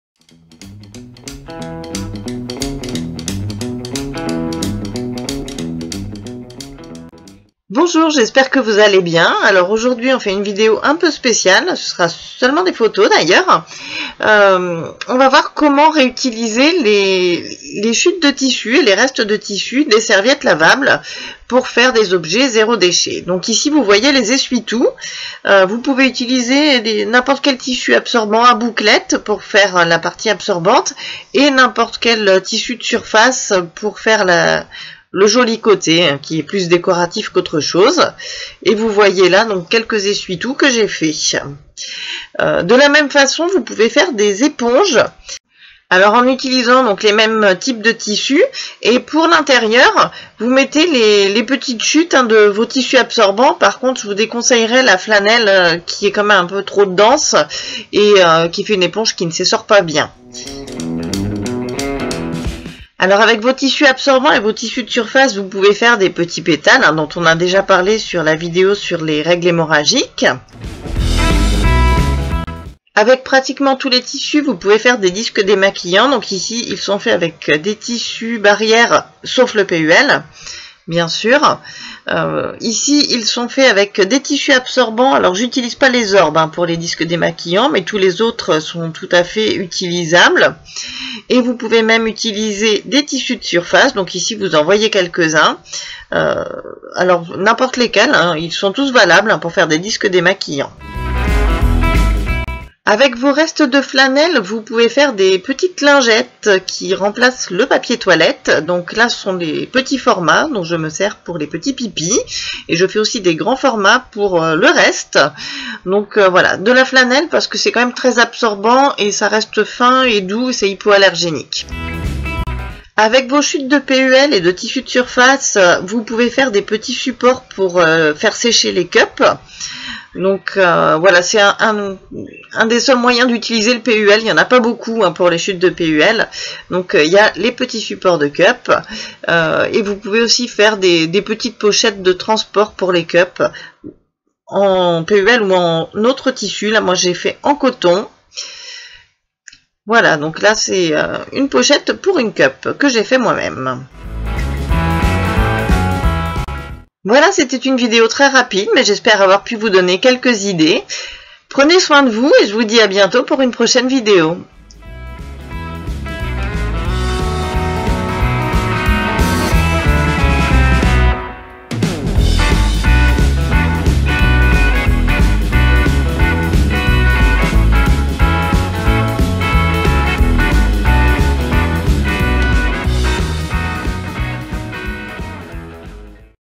Zéro déchet : utiliser les chutes de tissus des serviettes lavables
Quelques idées d'objets à réaliser avec vos chutes et restes de tissus de serviettes lavables pour un mode de vie zéro déchet.